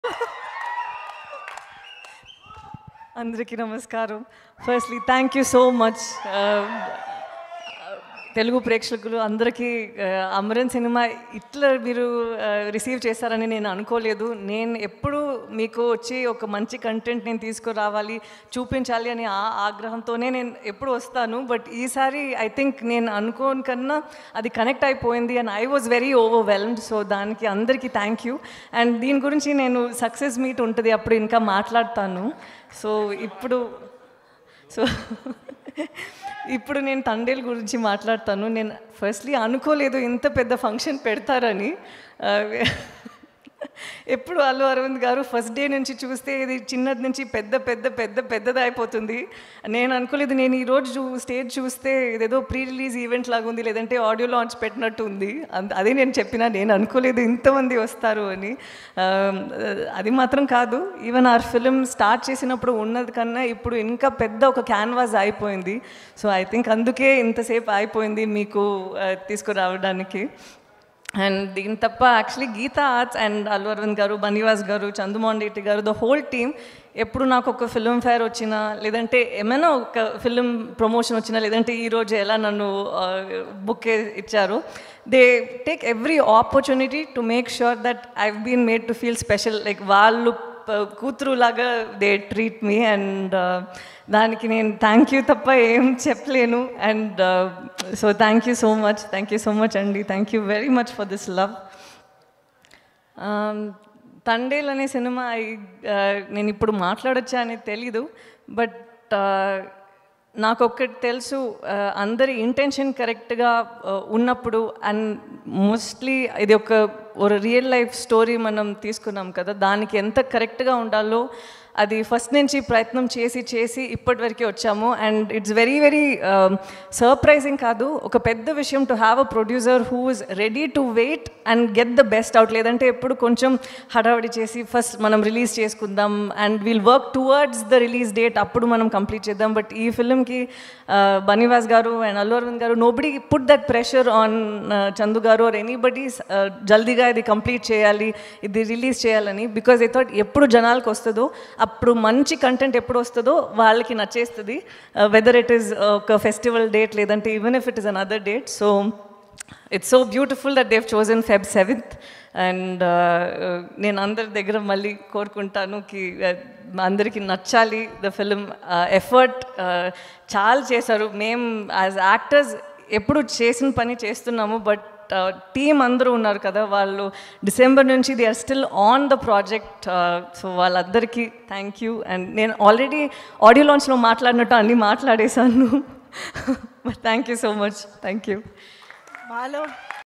Actress Sai Pallavi Speech @ Thandel Movie Press Meet | MS Talkies
Watch Actress Sai Pallavi Speech @ Thandel Movie Press Meet